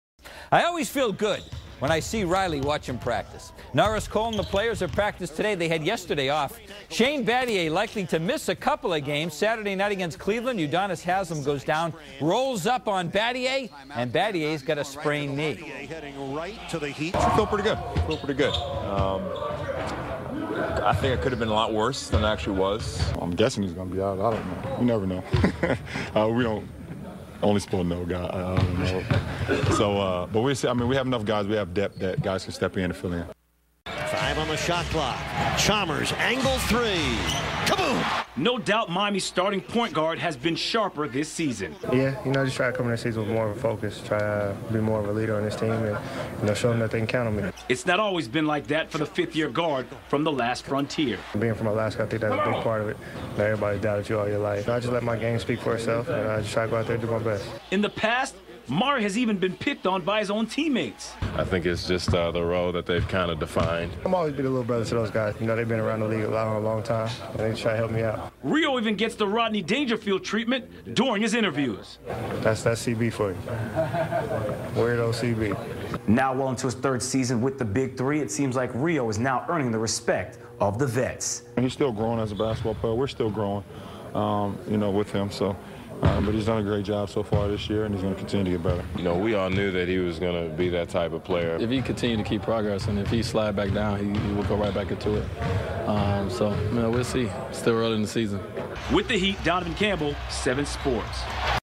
November 26, 2013 - WSVN - Miami Heat Practice (Battier Knee, Chalmers Improved Play this Season)
November 26, 2013 - WSVN - Miami Heat Practice (Shane Battier Knee, Mario Chalmers Improved Play this Season)